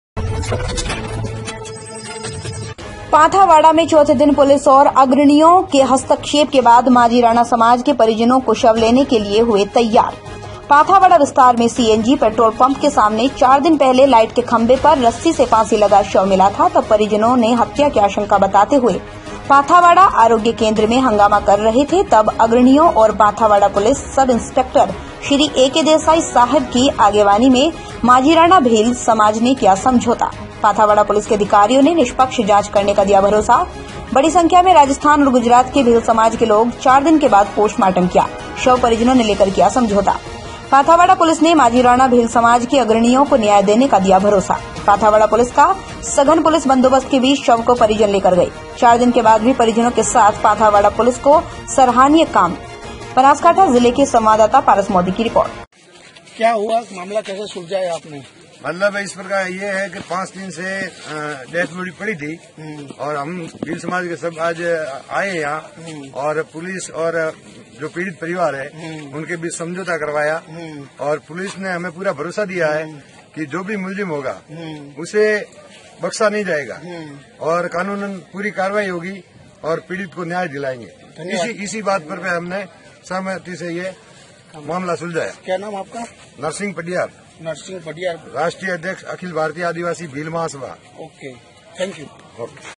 । Nonstop News । खबर बनासकांठा गुजरात से । Gyam News । 18 दिसंबर 2020 ।
Gyam national news बनासकांठा  17/12/2020

पांथावाडा मे चोथे दिन पुलिस और   अग्रणीओ के हस्तक्षेप के बाद माजीराणा समाज के परिजनों शव को लेने की लिए हुए तैयार |  पांथावाडा विस्तार में CNG  पेट्रोल पंप के सामने चार दिन पहले लाईट के खंबे पर रस्सी से फांसी लगा शव मिला था तब परिजनों हत्या की आशंका बताते हुए पांथावाडा आरोग्य केन्द्र मे हंगामा कर रहे थे तब अग्रणीओ ओर  पांथावाडा पुलिस सब ईस्पेक्टर श्री ऐ. के देसाई साहेब की आगेवानी मे माजीराणा भील  समाजने किया समजौता |पांथावाडा पुलिस के अधिकारियों ने निष्पक्ष जाँच करने का दिया भरोसा |   बडी संख्या में राजस्थान ओर गुजरात के भील समाज के लोग चार दिन के बाद पोस्टमार्टम किया शव परिजनों लेकर किया समजौता | पांथावाडा पुलिस ने माजीराणा भील समाज के अग्रणीओ को न्याय देने का दिया भरोसा |पांथावाडा पुलिस का संघन पुलिस बंदोबस्त के बीच शव को परिजनो लेकर गए | चार दिन के बाद भी परिजनों के साथ पांथावाडा पुलिस की सराहनीय कामगिरी |  बनासकांठा जिले के संवाददाता पारस मोदी की रिपोर्ट |